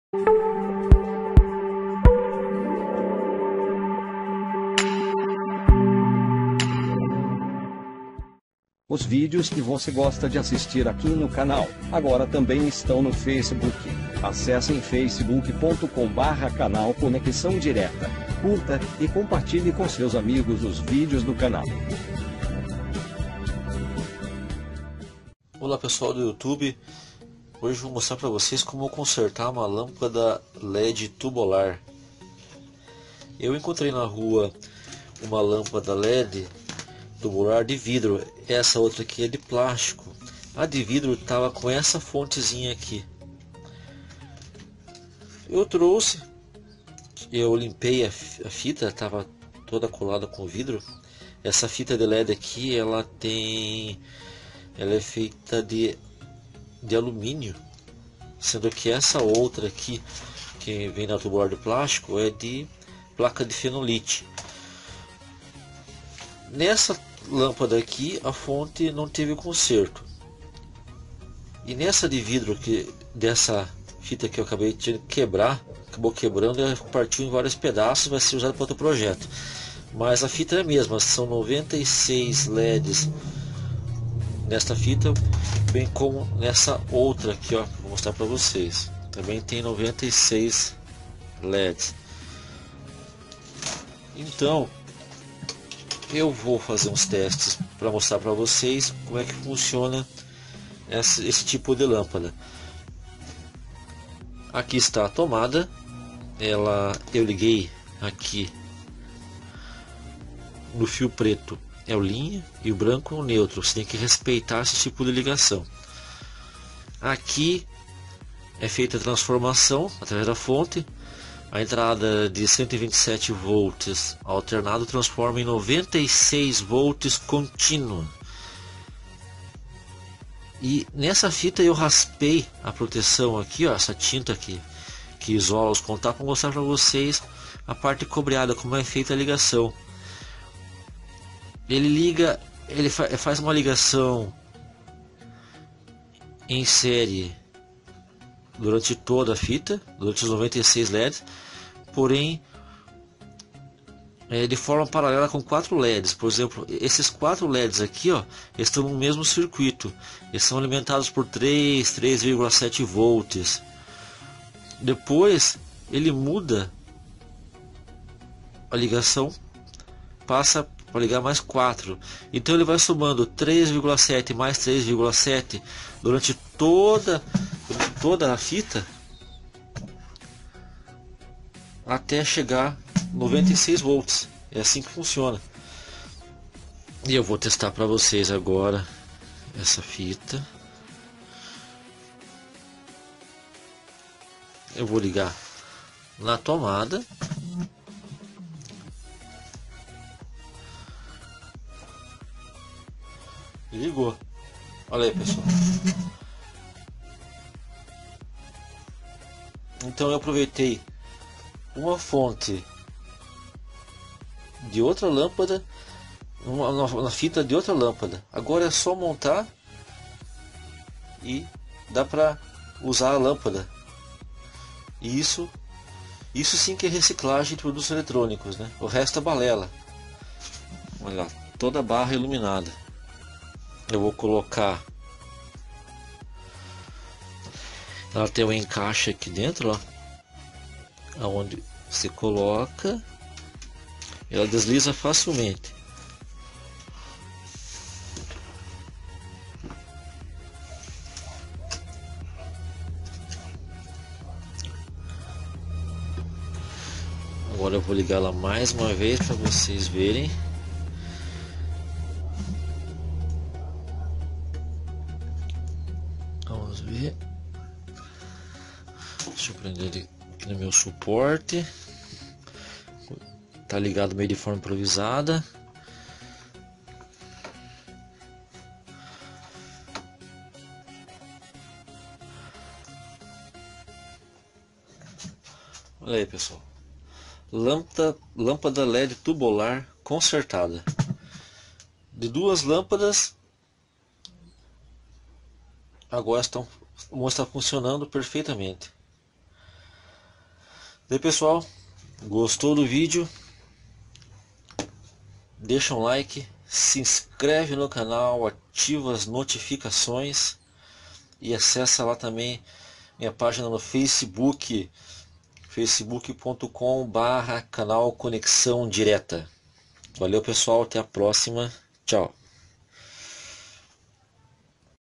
✅ Como Consertar Lâmpada LED Tubular. Passo a Passo. Não Jogue Fora!
Neste vídeo eu mostro como consertar uma lâmpada de LED do tipo tubolar desde os leds até os possíveis defeitos da fonte interna. É um vídeo com bastante informação de proveito para nossas vidas.

📱Controle Remoto Universal
🎚Multímetro Digital com Capacímetro ➤
🖊Ferro de Solda 60 W ➤
💡Lampada led Gigante
🥛Jarra Eletrica
💵TOP Itens na Amazon